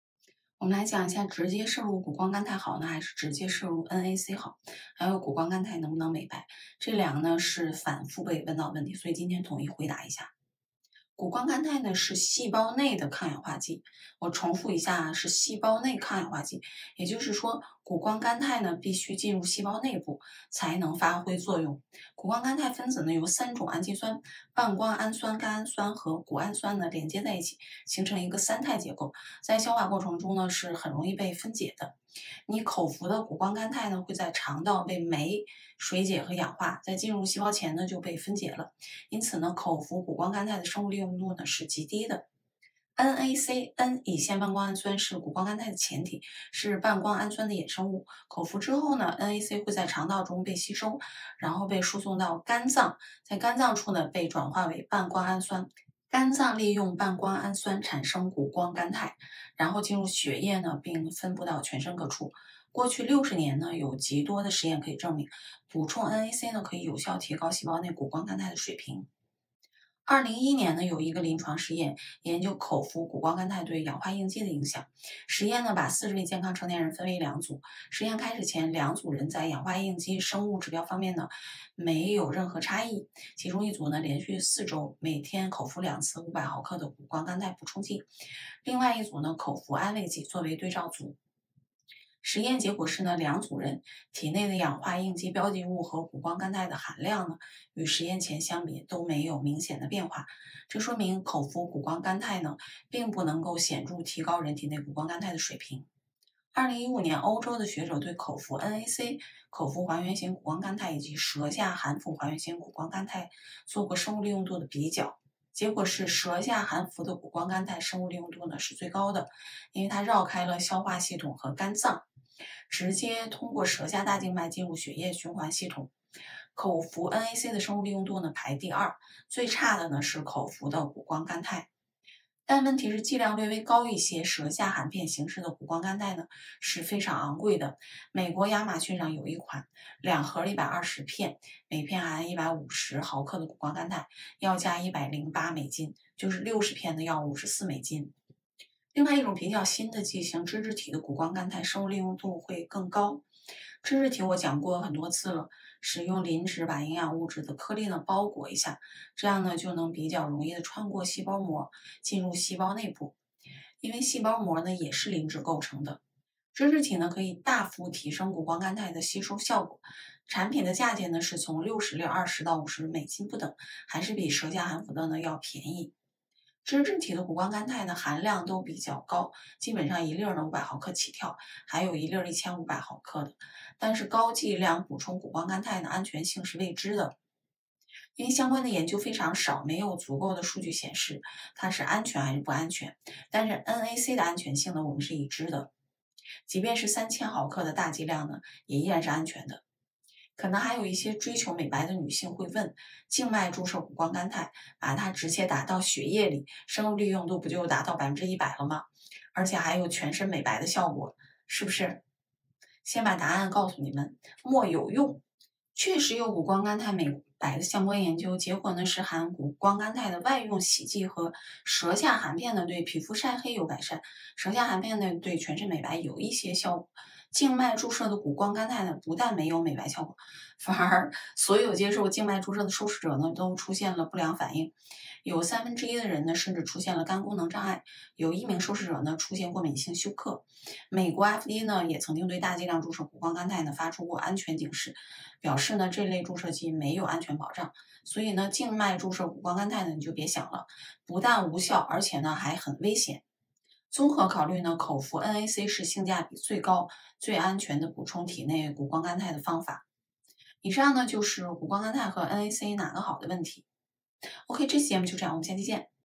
直接摄入谷胱甘肽好还是直接摄入NAC好 ? 谷胱甘肽能不能美白？
▼ 影片目录 ▼

00:00 谷胱甘肽还是NAC

03:20 谷胱甘肽能美白吗